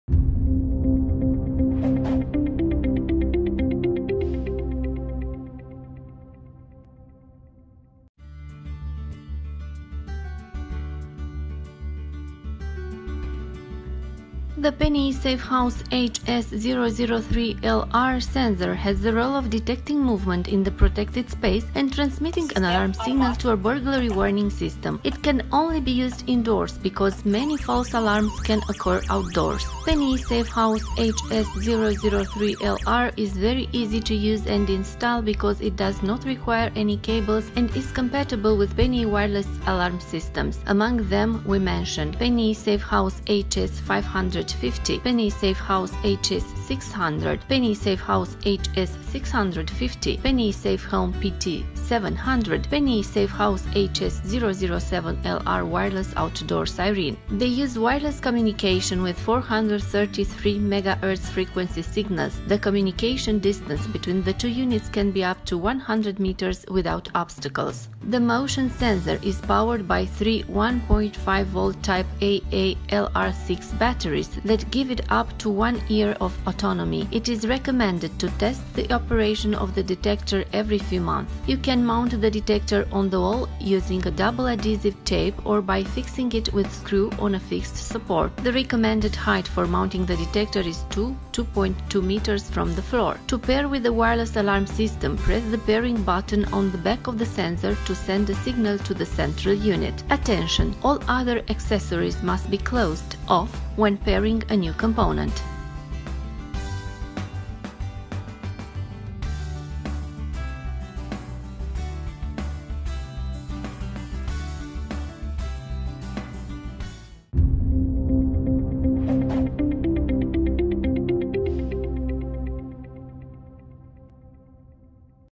Bezprzewodowy czujnik ruchu PIR PNI SafeHouse HS003LR PL
Czujnik PNI SafeHouse HS003LR pełni funkcję wykrywania ruchu w chronionej przestrzeni i przekazywania sygnału alarmowego do systemu sygnalizacji włamania. Może być używany tylko w pomieszczeniach, ponieważ wiele fałszywych alarmów może wystąpić na zewnątrz.
PNI SafeHouse HS003LR jest bardzo łatwy w obsłudze i instalacji, ponieważ nie wymaga żadnych kabli i jest kompatybilny z bezprzewodowymi systemami alarmowymi PNI. Wśród nich wymieniamy:
• Kryjówka PNI HS550
• Kryjówka PNI HS600
• Kryjówka PNI HS650
• PNI SafeHome PT700
• Bezprzewodowa syrena zewnętrzna PNI SafeHouse HS007LR.
Wykorzystują komunikację bezprzewodową z sygnałami o częstotliwości 433 MHz. Odległość komunikacyjna między dwoma jednostkami może wynosić do 100 metrów bez przeszkód.
Czujnik ruchu jest zasilany trzema bateriami 1,5 V typu AA (LR6), które zapewniają do 1 roku autonomii. Zaleca się testowanie działania czujki co kilka miesięcy.
Czujkę można zamontować na ścianie za pomocą taśmy dwustronnie klejącej lub mocując ją śrubami na stałym wsporniku. Zalecana wysokość montażu czujki to 2-2,2 m od podłogi.
Aby sparować z bezprzewodowym systemem alarmowym, naciśnij przycisk parowania z tyłu czujnika, aby wysłać sygnał do jednostki centralnej.
Uwaga, wszystkie inne akcesoria muszą być zamknięte (OFF) podczas parowania nowego komponentu.